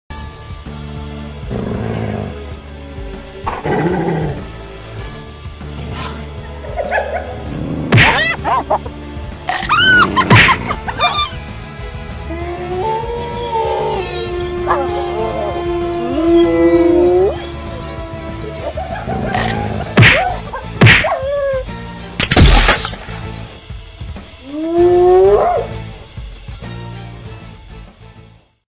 Stop Motion Animal Battles: Wolf vs. Hyena (read description)
In Hyena Hideout, Claw thought he was easily gonna conquer Kyo and take over her world. What he didn't know was that he was actually digging his own grave.

Also, for those of you wondering, the two combatants are an alpha male Timber wolf and an alpha female Spotted hyena respectively (just in case it wasn't clear).